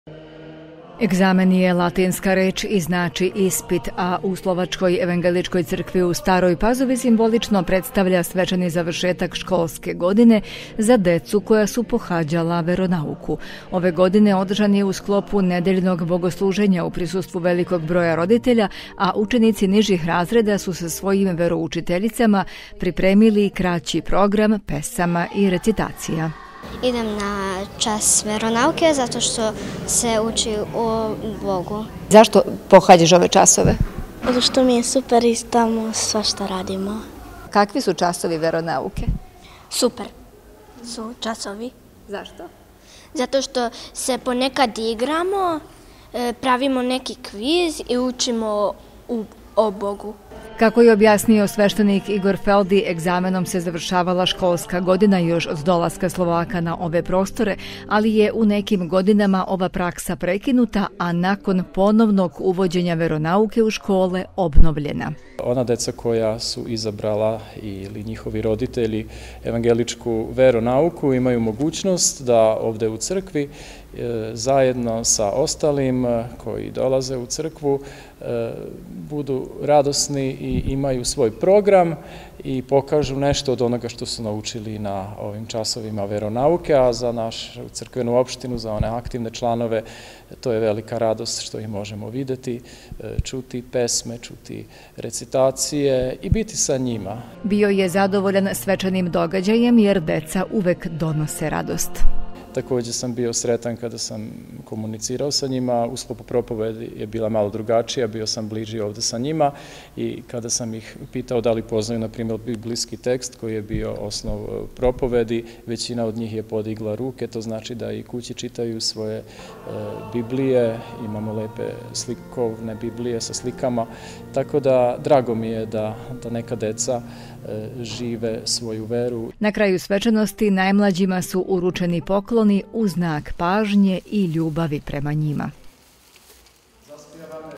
Svečani završetak nastave verounauke
U Slovačkoj evangeličkoj crkvi u nedelju je u okviru nedeljne službe održan i egzamen, polaganje završnog ispita učenika koji su pohađali verounauku u ovoj školskoj godini.